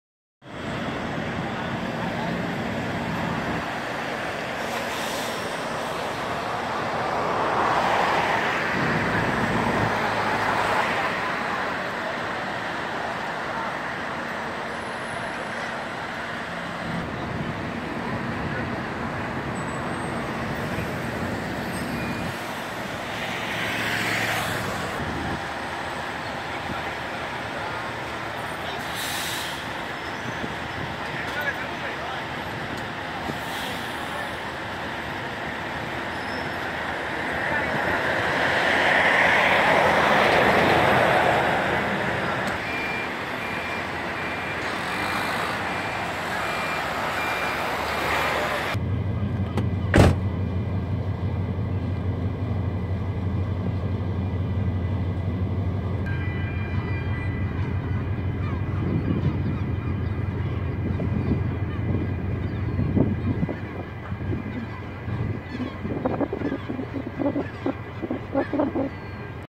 Segunda jornada de paros en el puerto de Algeciras
Vídeo: Erasmo Fenoy